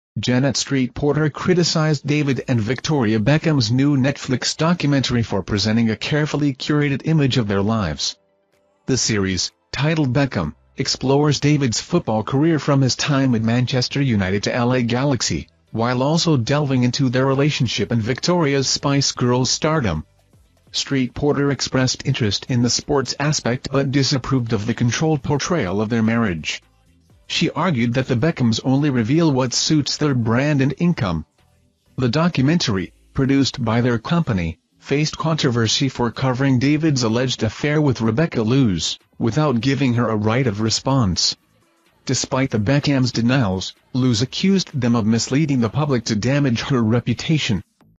David and Victoria Beckham slammed for showing 'what they want us to see' in Netflix doc
Janet Street-Porter has hit out at David and Victoria Beckham for the "manicured" portrayal of their lives in their new Netflix documentary.
JanetStreet-Porter#Netflix#VictoriaBeckham#